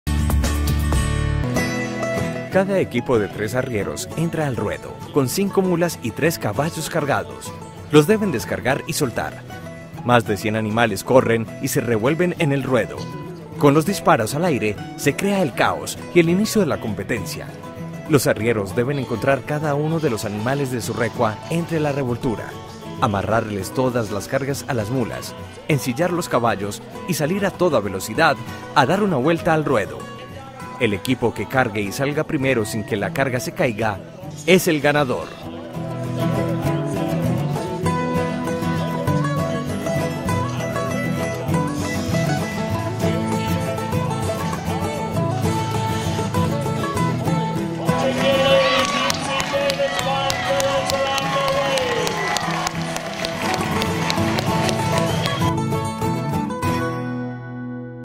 Pack Scramble - La revoltura
Pack Scramble (La revoltura) Una prueba de habilidad de los arrieros en Mule Days